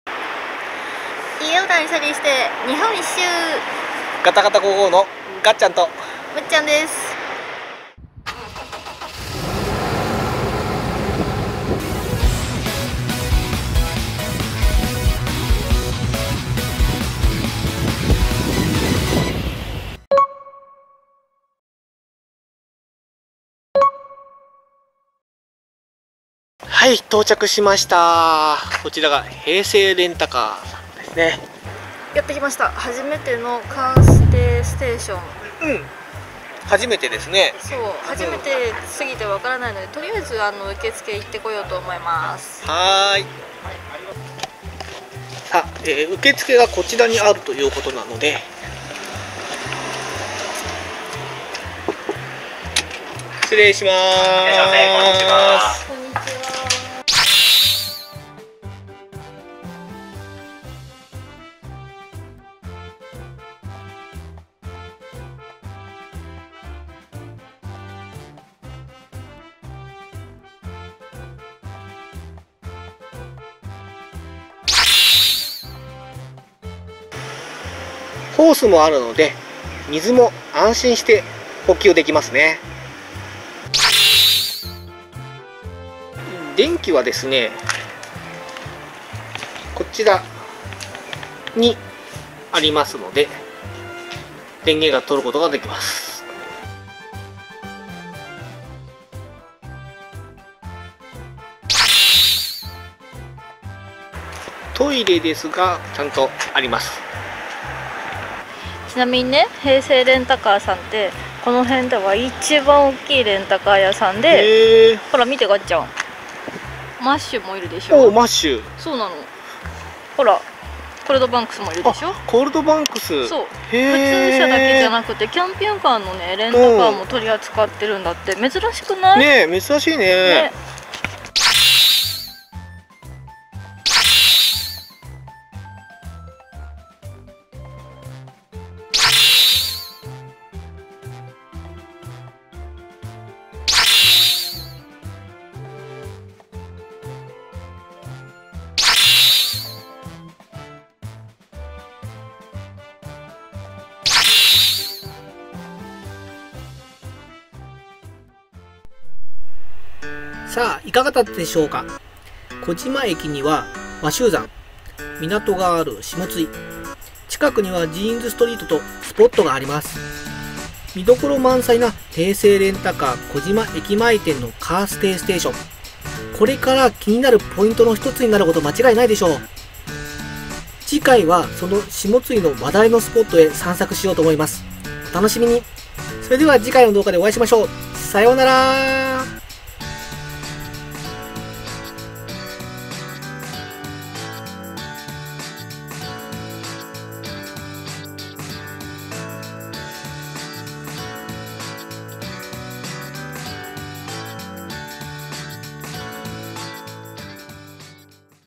キャンピングカーで車中泊スポット・カーステイステーションで車中泊してみた【バンライフ】
岡山県倉敷市の平成レンタカー児島駅前店にあるカーステイステーション（CarstayStation）を実際に使ってみました。
基本料金1000円　外部電源追加料金500円
安いですよね！
詳細はカーステイ様ＨＰにてご確認下さい。

▼ガタガタGOGOファンクラブを登録する

▼撮影機材提供
渡邉カズヨシ@bcj70ut2hSBV0iF